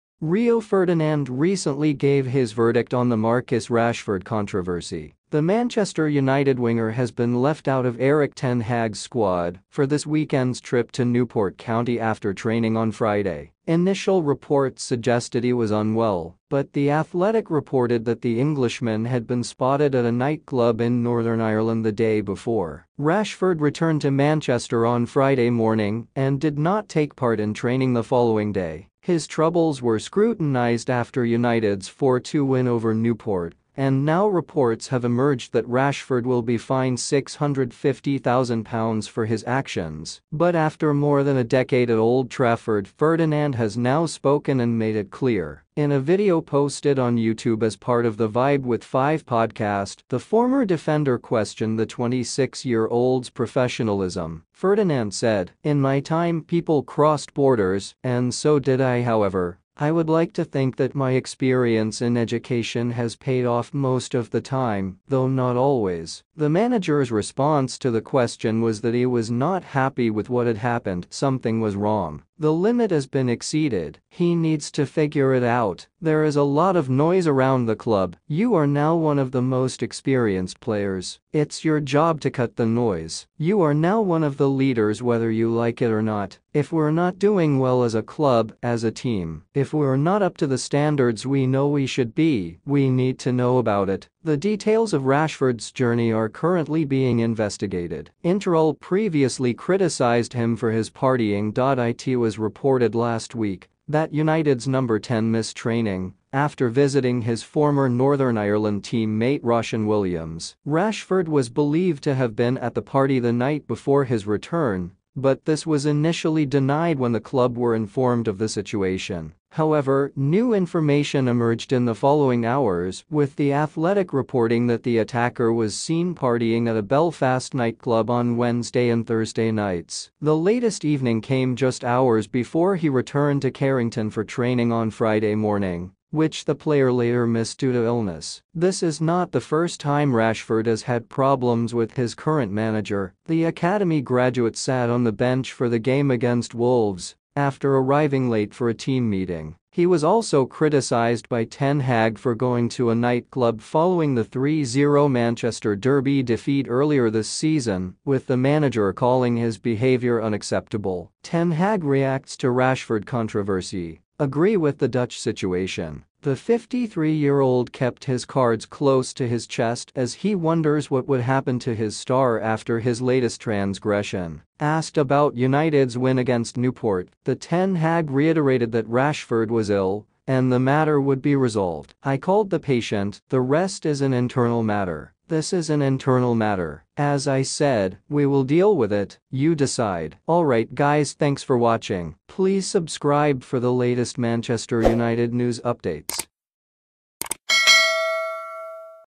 Rio Ferdinand criticises Rashford for missing training and spotted in nightclub instead
Inside this video, Rio Ferdinand slams Marcus Rashford for clubbing on eve of United's game against Newport County. He called Rashford's attitude unprofessional and he had to be sanctioned.

Hi there, welcome to UNITED VIBES!! Here we report Manchester United latest news updates, transfer updates, match preview, match review, highlights and match analysis!!